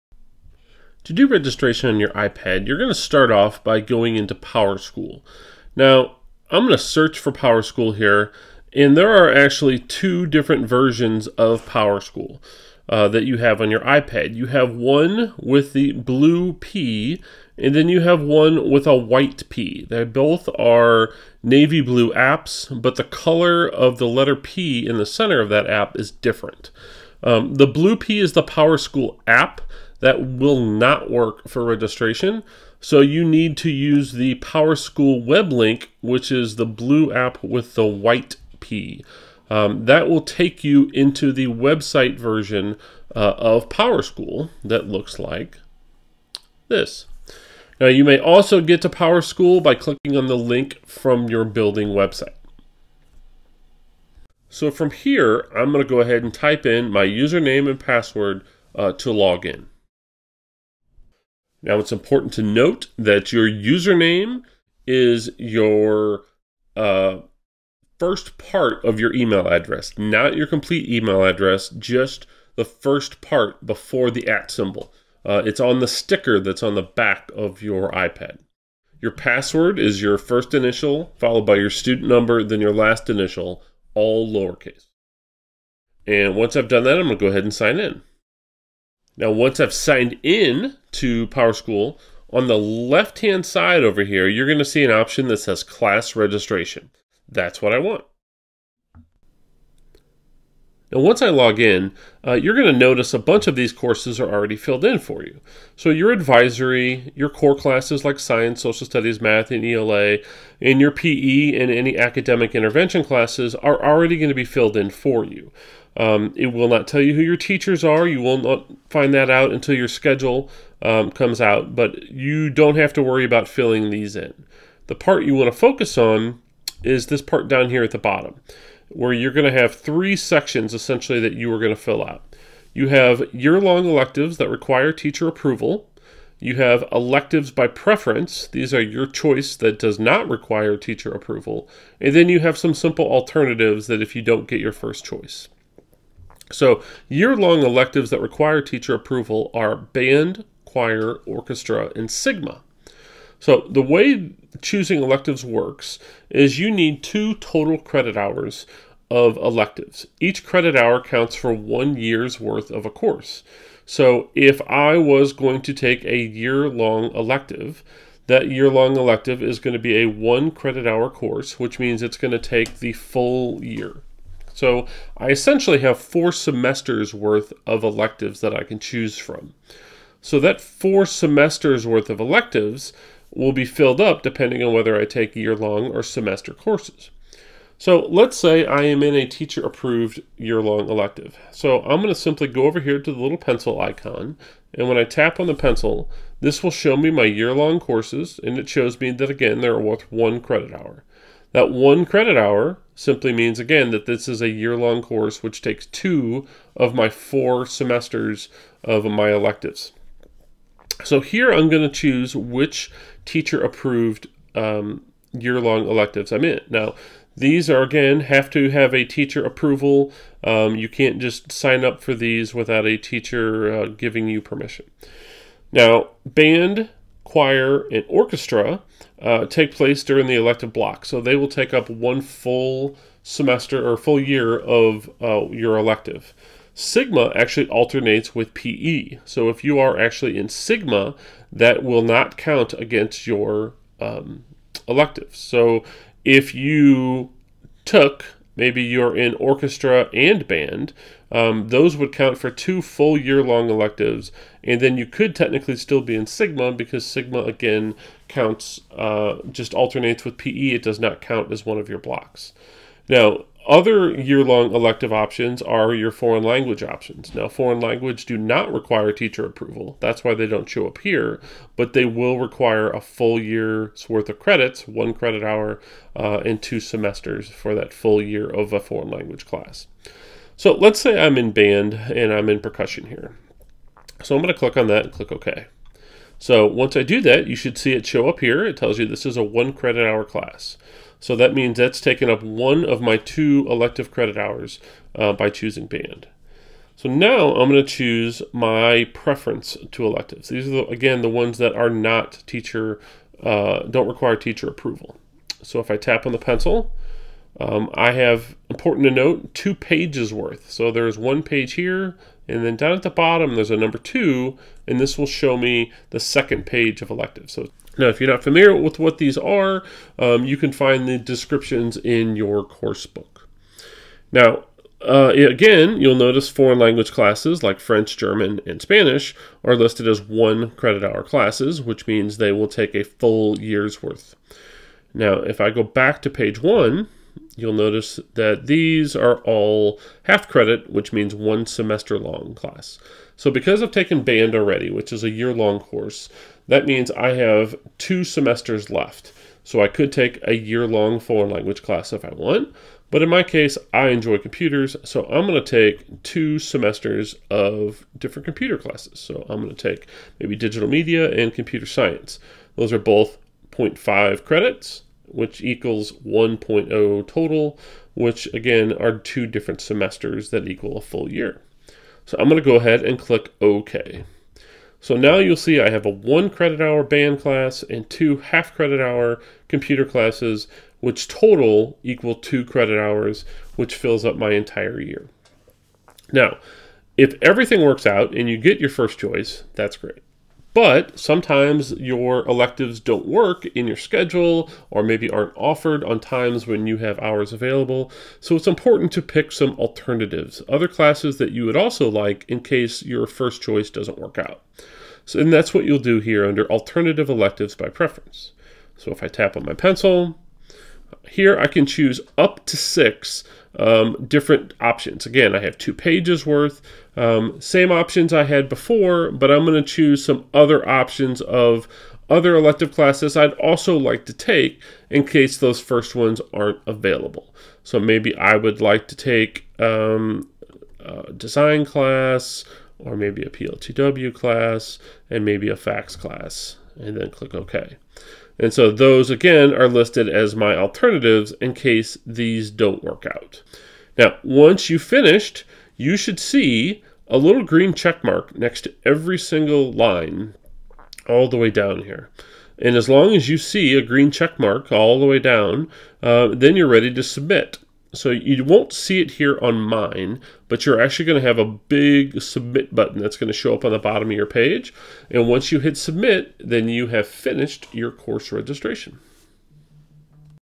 Course Registration with PowerSchool
This video shows how Middle School students will register for classes online using PowerSchool.